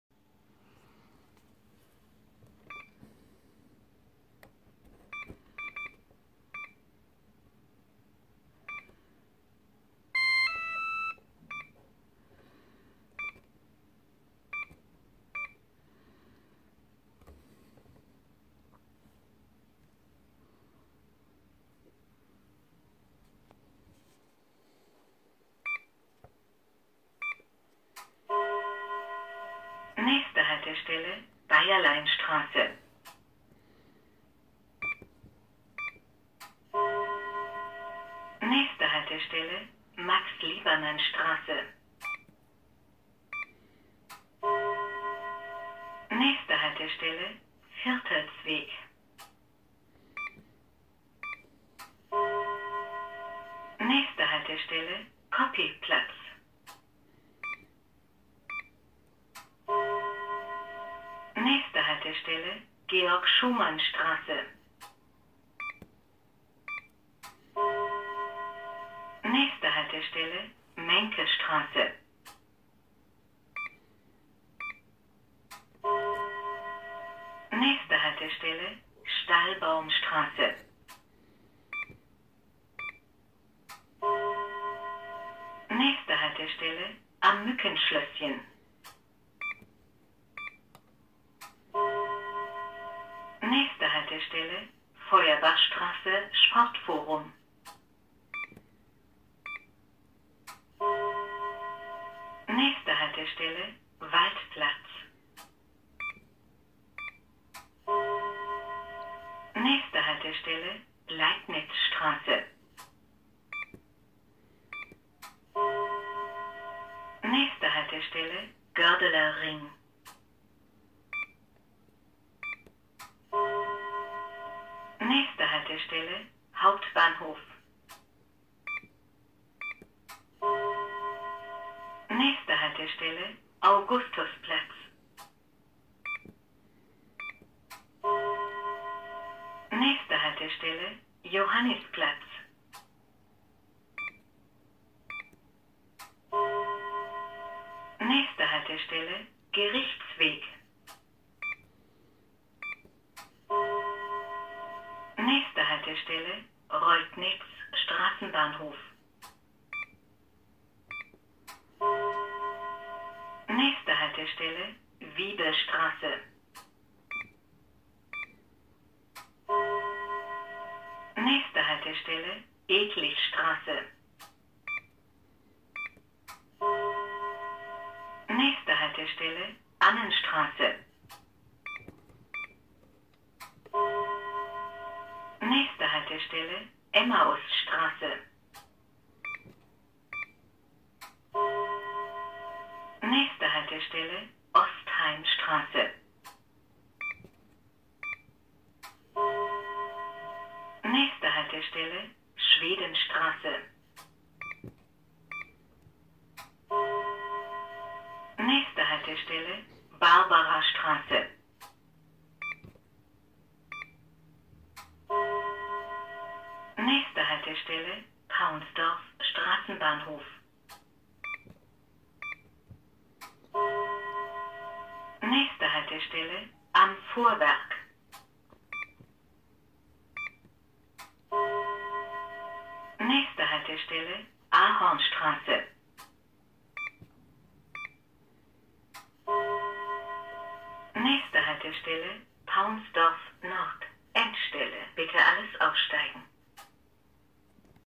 LVB-Ansagen von 1993 Linie 6 nach Paunsdorf-Nord
LVB-Ansagen von einem meister SAG-PCM.
Dieses Ansagegerät mit dem IBIS1 war in einem der ersten modernisierten Tatras (T4D-M) von 1992 bis etwa 1995 eingebaut.
Auf dem Ansagegerät sind nur die Linien 6 (Paunsdorf-Nord - Gohlis) und 8 (Paunsdorf-Nord - Lausen) gespeichert, weil die ersten T4D-M im Straßenbahnhof Paunsdorf beheimatet wurden.
Auf dem IBIS1 ist auch schon die Streckenverlängerung nach Sommerfeld eingespeichert, die Ansagen wurden leider nicht angepasst.

Hier zu hören sind die Ansagen der Linie 6 von Gohlis Landsberger Straße nach Paunsdorf-Nord.

Vielen Dank an die Verkehrsgesellschaft Frankfurt am Main (VGF) für das zur Verfügung stellen dieser IBIS-ATM!!